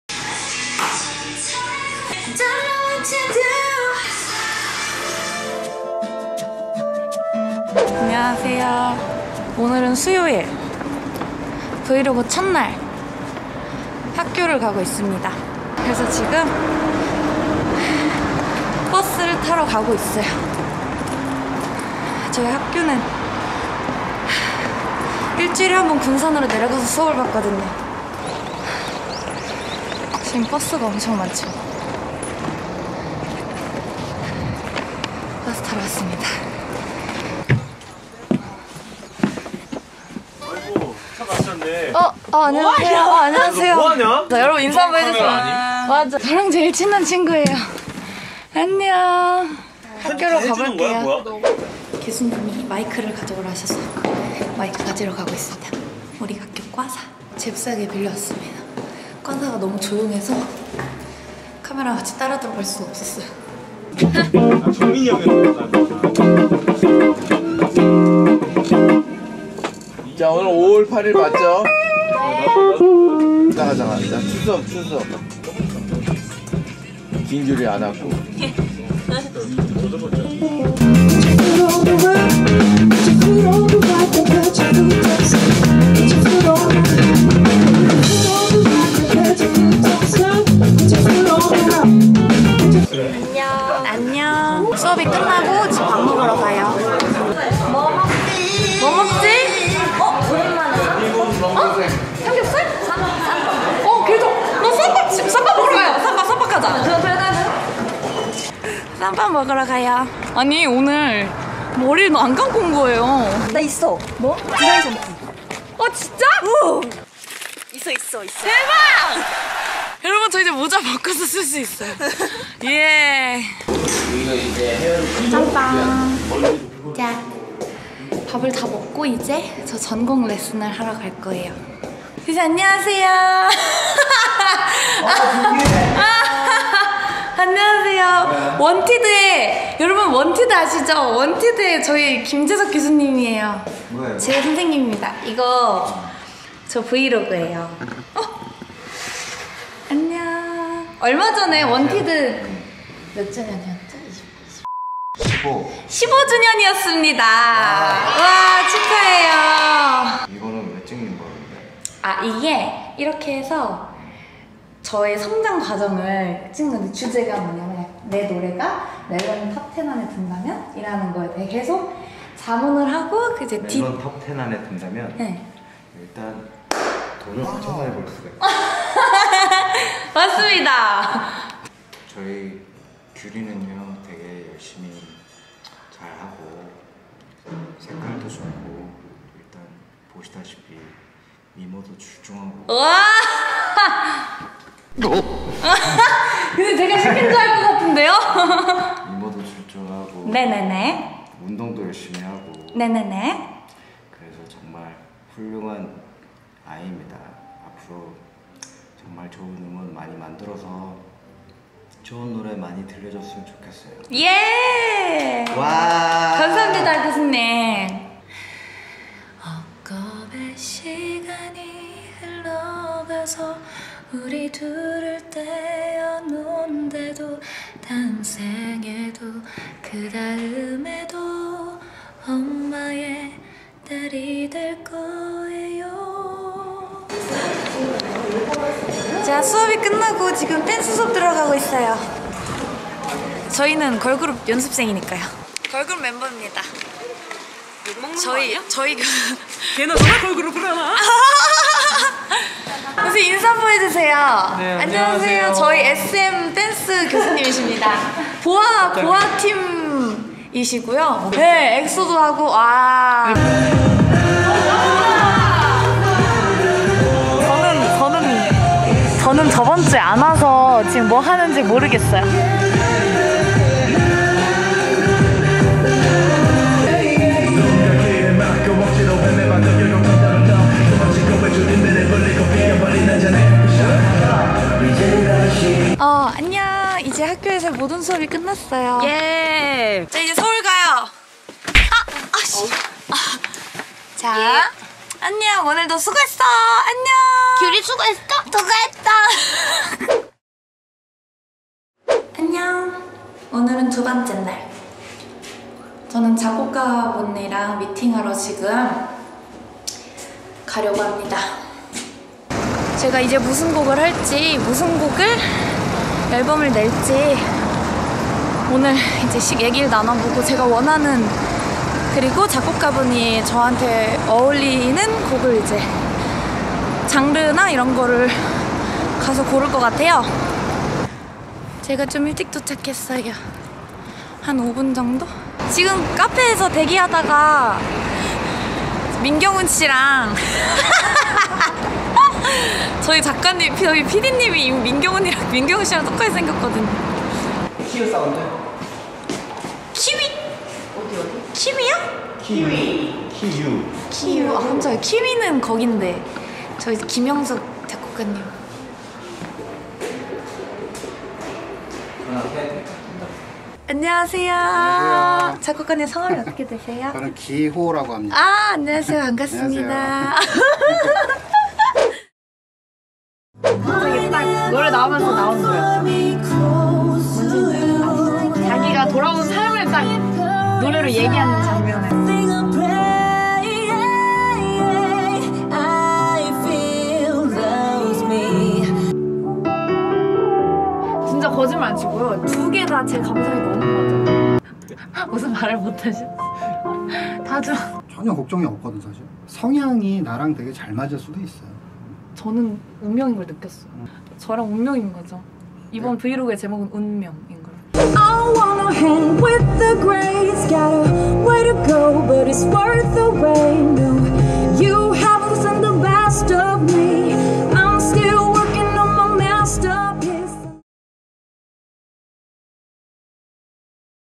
VLOG #01. 깨발랄 + 러블리 김규리 리얼 일상! 실음과 학생은 하루 종일 뭘 할까?
[ KOR / ENG ]

김규리의 잔망 터지는 학교생활 밀착 브이로그

Lovely Gyuri's daily life!
What does a music student do all day?

Gyuri's playful school life V-log